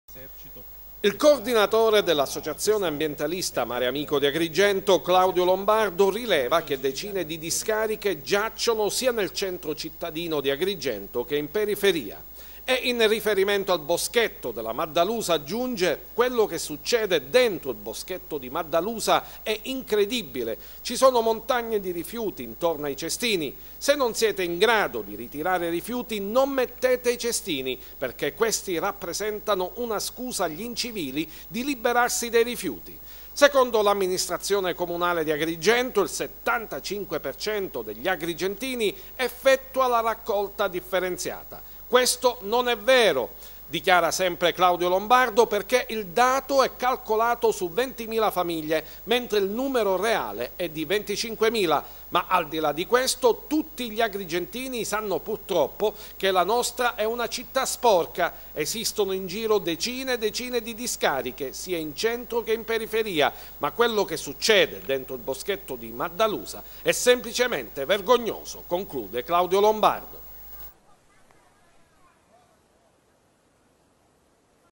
Mareamico: "La vergogna delle discariche al boschetto Maddalusa" [STUDIO 98]
Il coordinatore dell’associazione ambientalista MareAmico di Agrigento, Claudio Lombardo, rileva che decine di discariche giacciono sia nel centro cittadino di Agrigento che in periferia. E, in riferimento al boschetto della Maddalusa, aggiunge: “Quello che succede dentro il boschetto di Maddalusa è incredibile: ci sono montagne di rifiuti intorno ai cestini. Se non siete in grado di ritirare i rifiuti, non mettete i cestini. Perchè questi rappresentano una scusa agli incivili di liberarsi dei rifiuti”.